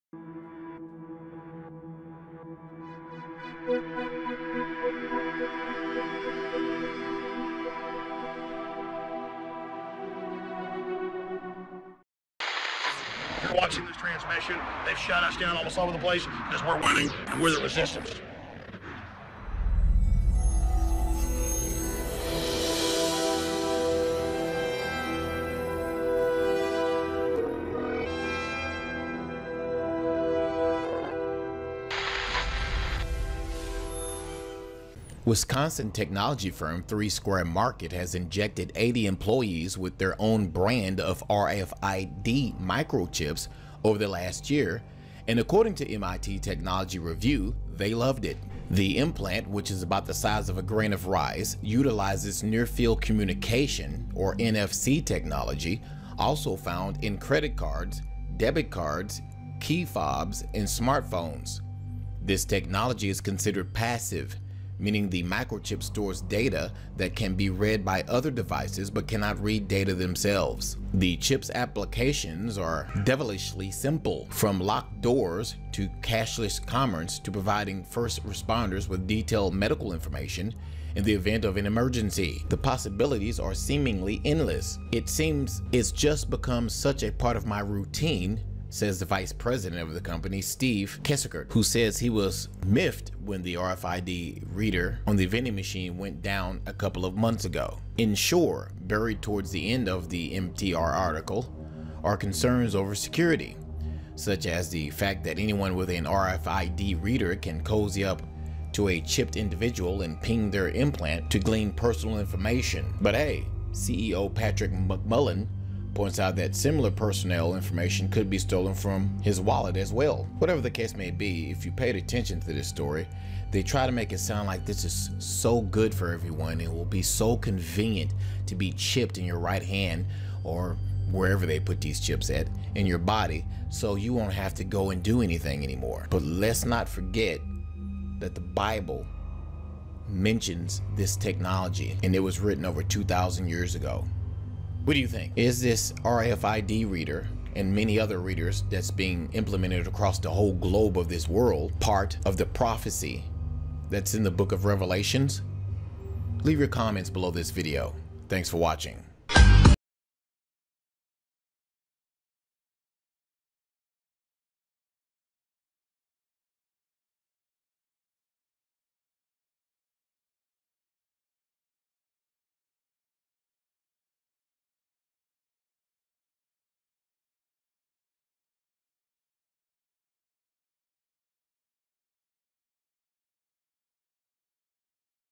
Wisconsin Company Injected RFID Microchips Into Hands Of Employees
Wisconsin technology firm Three Square Market has injected 80 employees with their own brand of RFID microchips over the last year, and according to MIT Technology Review, “they love it.”The implant, which is about the size of a grain of rice, utilizes Near Field Communication (NFC) technology, also found in credit cards, debit cards, key fobs, and smartphones. This technology is considered “passive,” meaning the microchip stores data that can be read by other devices but cannot read data themselves.

The chip’s applications are devilishly simple; from locked doors, to cashless commerce, to providing first responders with detailed medical information in the event of an emergency – the possibilities are seemingly endless.

“It’s just become such a part of my routine,” says VP of finance Steve Kassekert, who says he was miffed when the RFID reader on the vending machine went down a couple of months ago.

And sure, buried towards the end of the MTR article are concerns over security – such as the fact that anyone with an RFID reader can cozy up to a chipped individual and ping their implant to glean personal information. But hey – CEO Patrick McMullan points out that “similar personal information could be stolen from his wallet, too.”